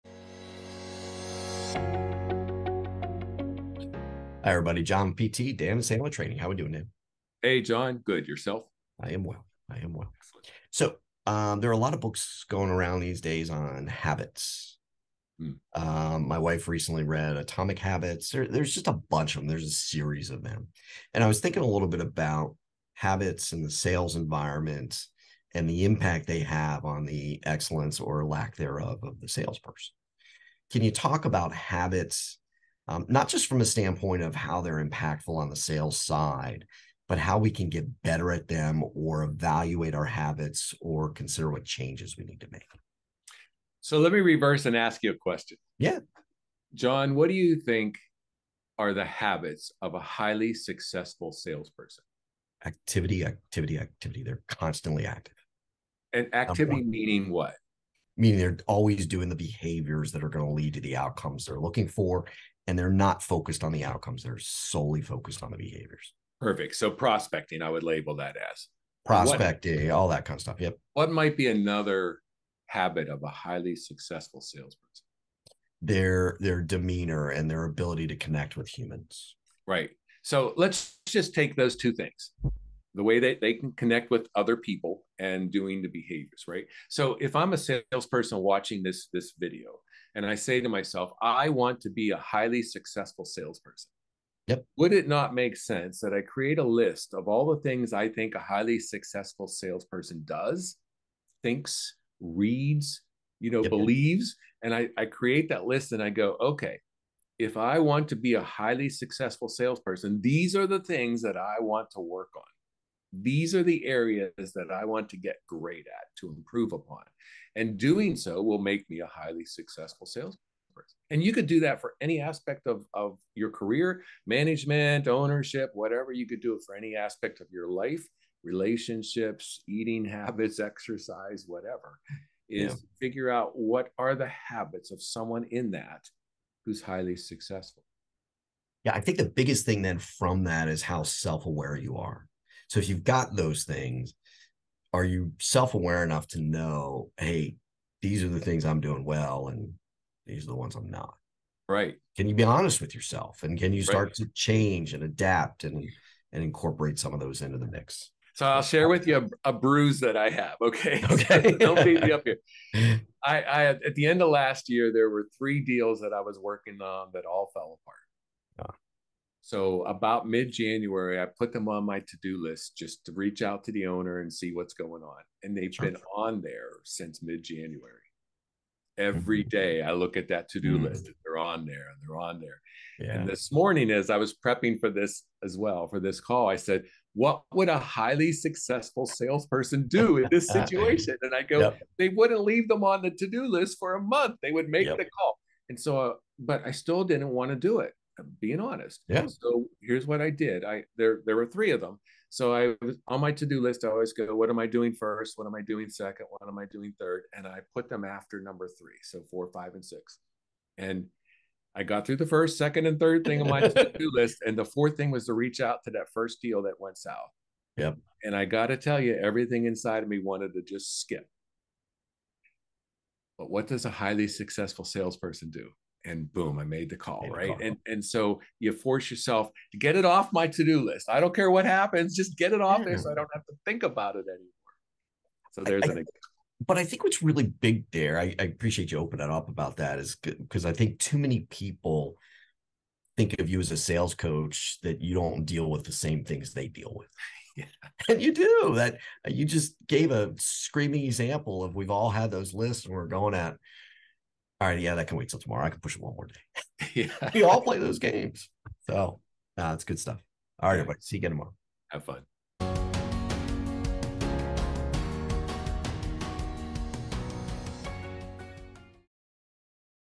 PT Asks Dan: Is excellence a habit in sales?
Dan and I discuss habits and how they impact sales people. Watch now.